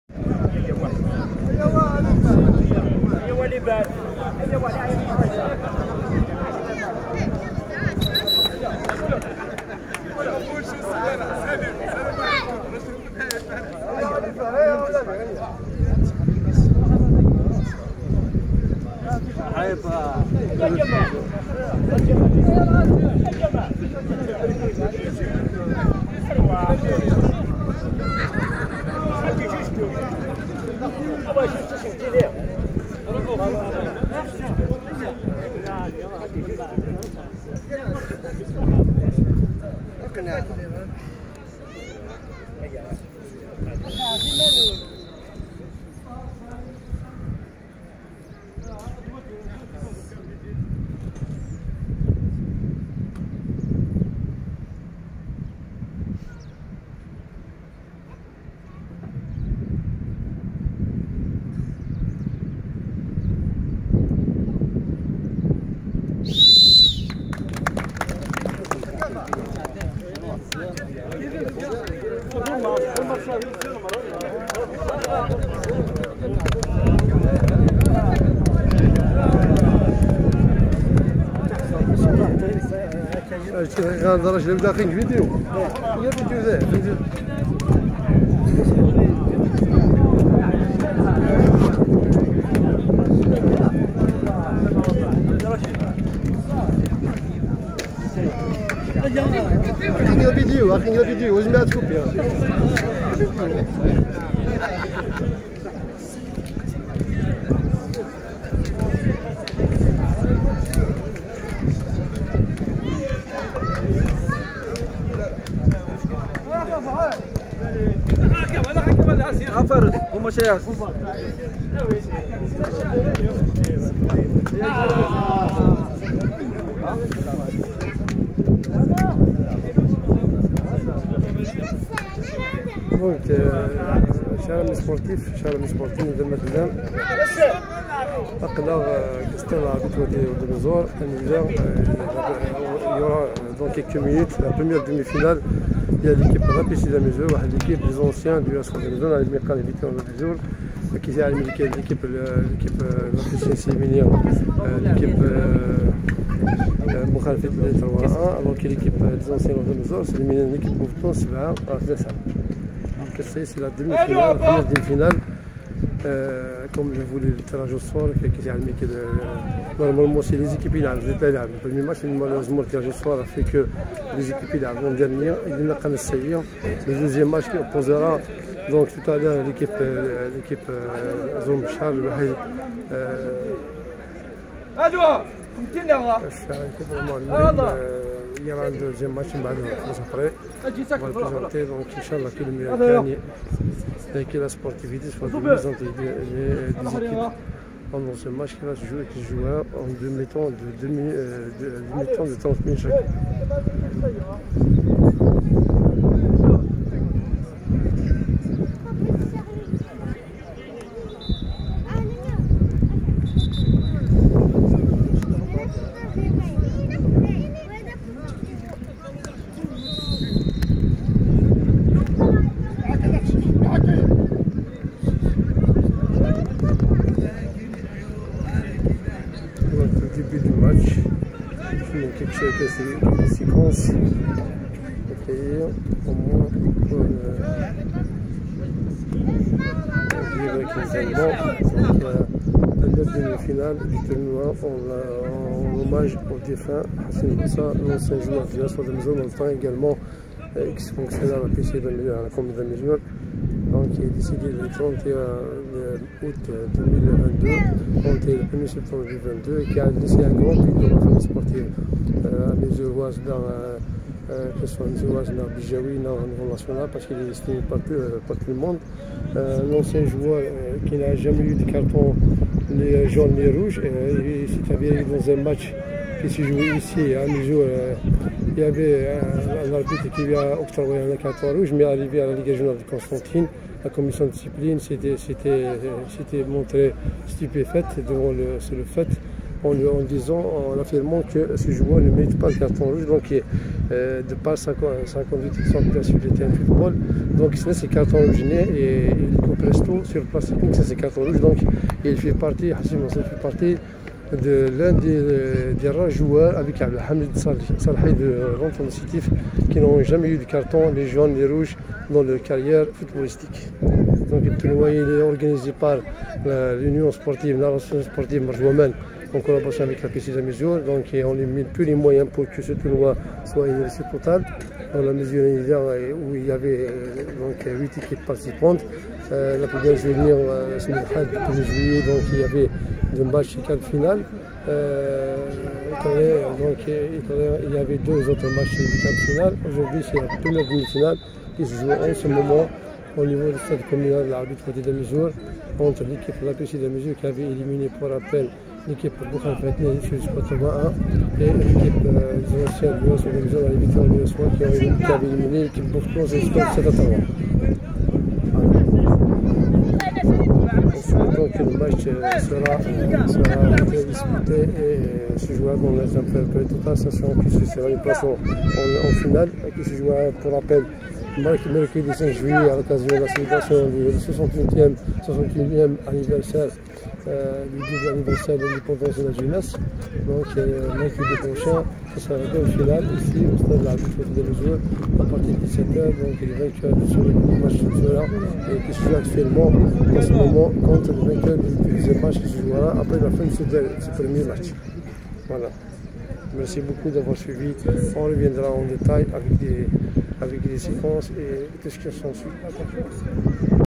Amizour.Tournoi en hommage au défunt Manseur Hocine. Demi-finale: APC Amizour 1- Vétérans USOA- 2
Amizour.Tournoi en hommage au défunt Manseur Hocine.

2e match des Demi-finales
APC Amizour 1- Vétérans USOA- 2